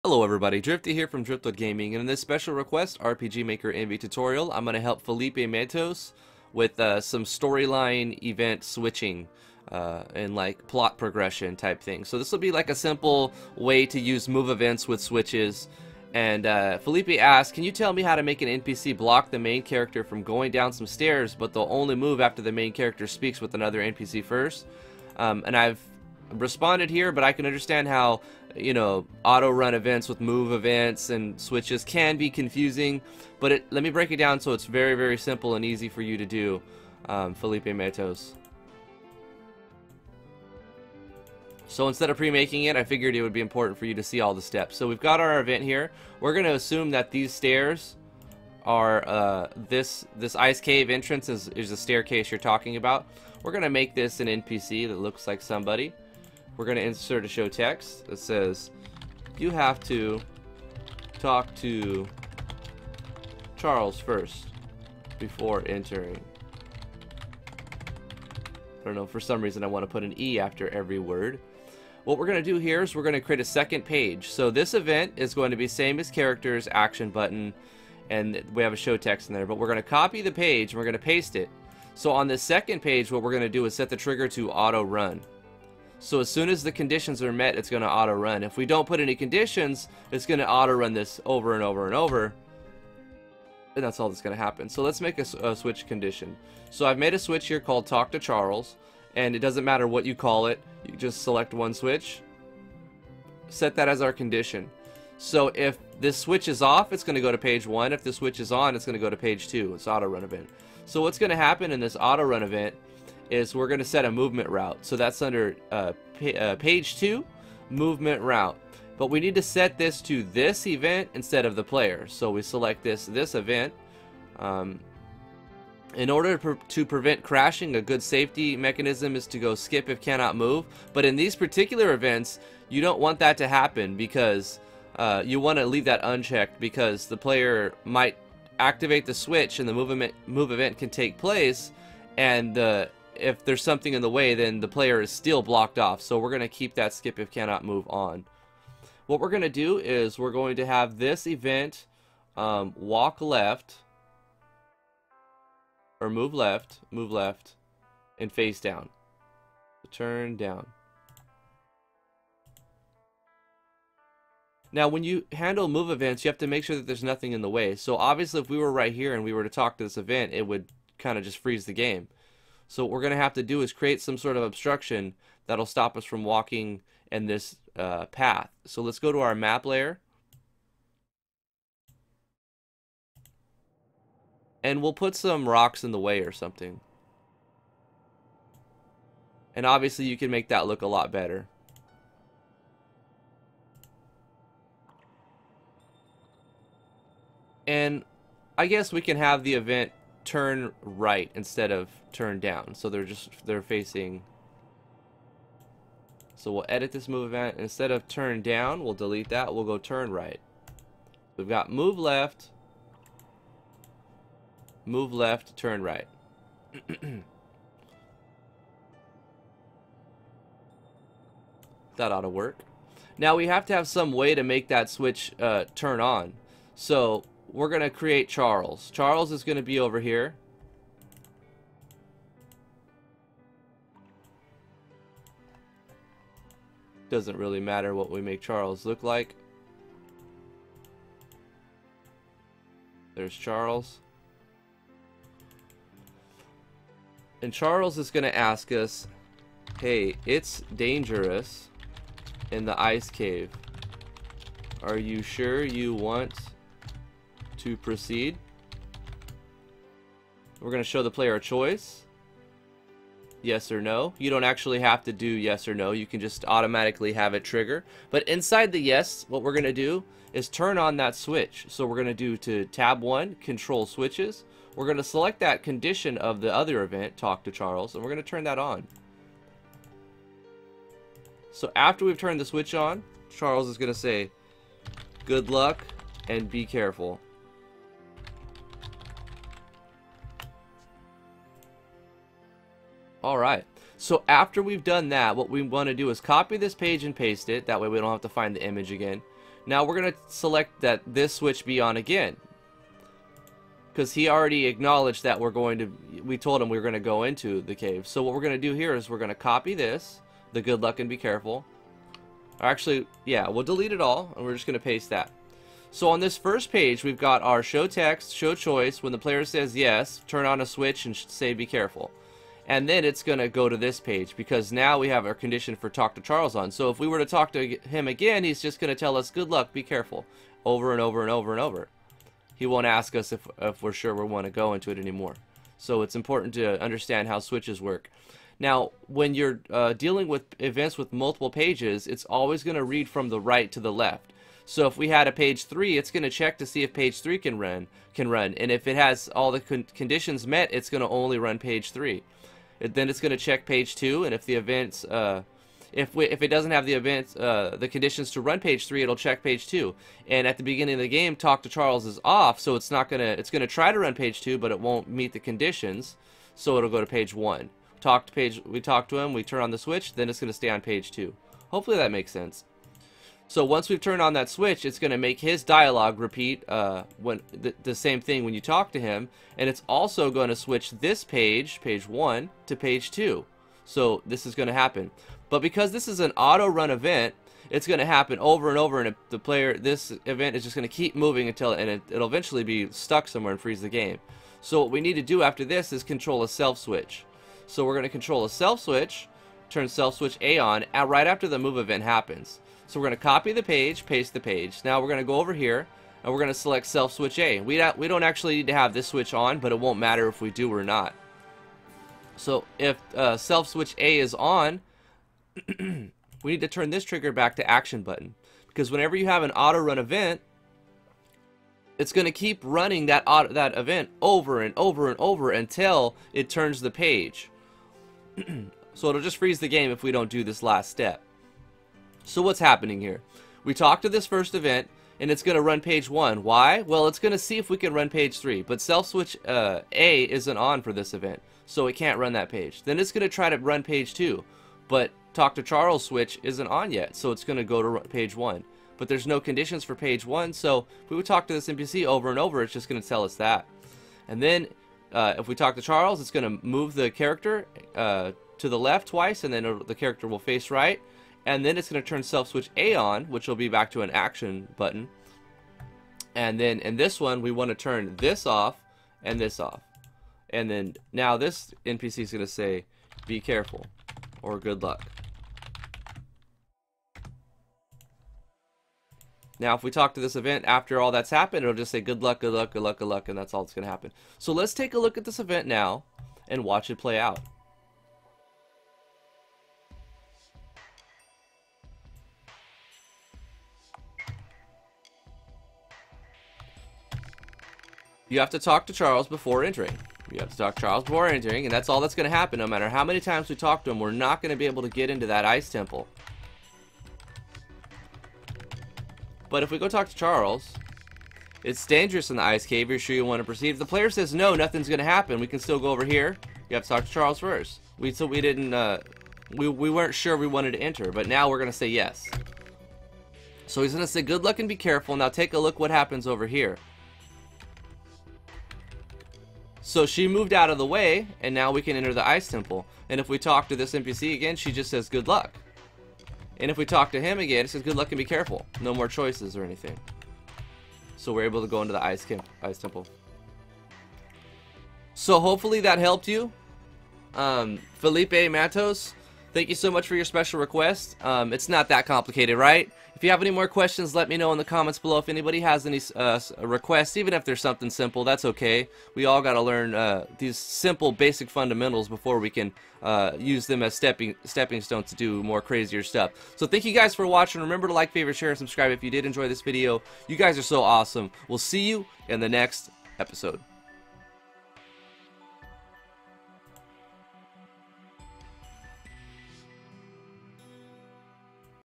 RPG Maker MV Tutorial Storyline Switches Are Easy
Hello everyone, in this special request RPG Maker MV tutorial I'll show you how to use move events, autorun events, and switch handling in one sequence. This is a very easy process but can go wrong in many ways. Hopefully this will clear up any confusion you may have with the order of things that have to happen.